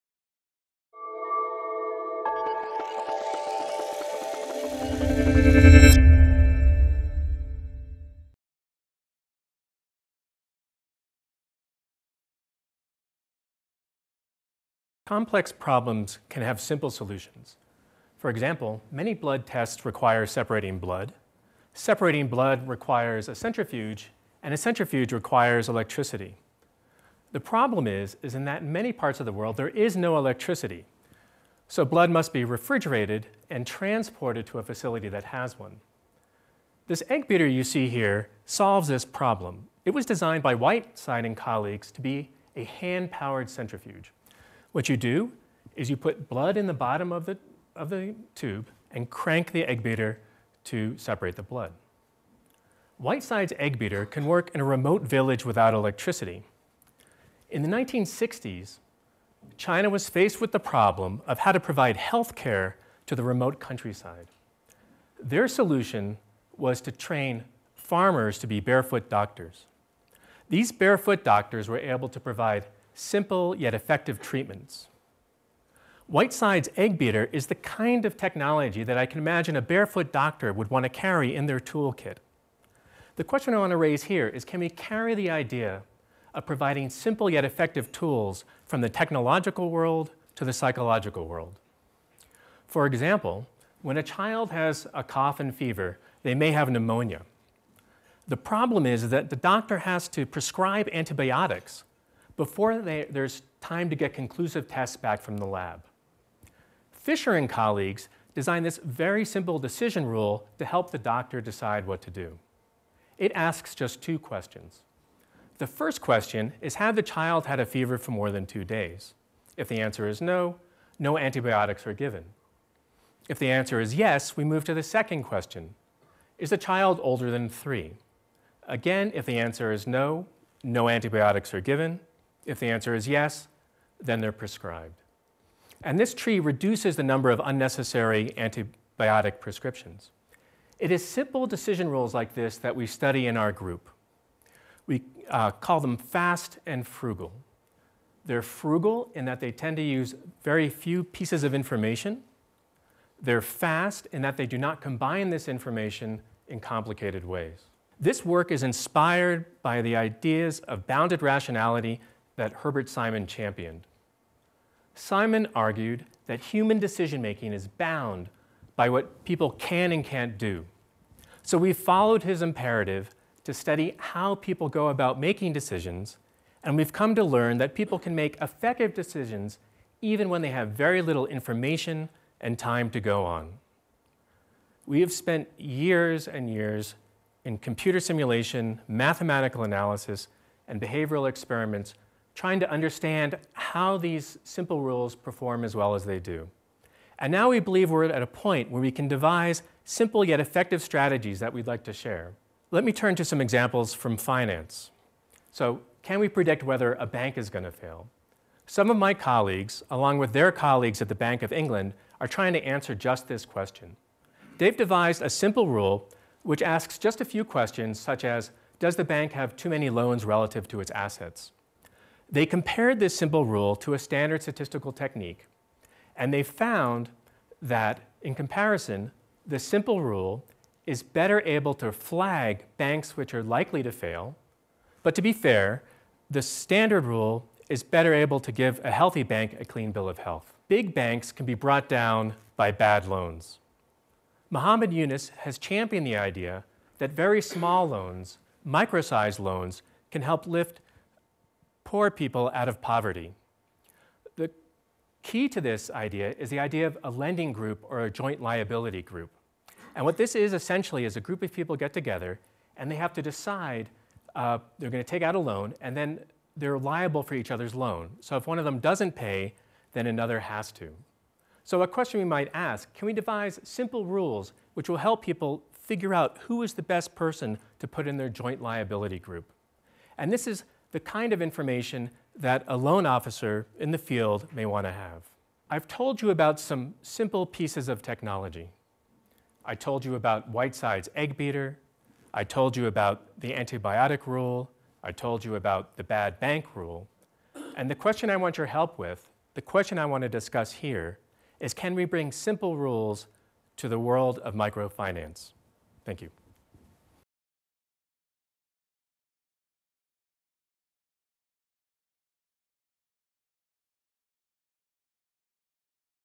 IdeasLab 2014 - Lael Schooler - Simple Rules for Complex Problems
From barefoot doctors storing blood safely without electricity, to ways to predict if a bank will fail, Lael Schooler from Max Planck Institute for Human Development describes how simple rules can help people can make "fast and frugal" decisions.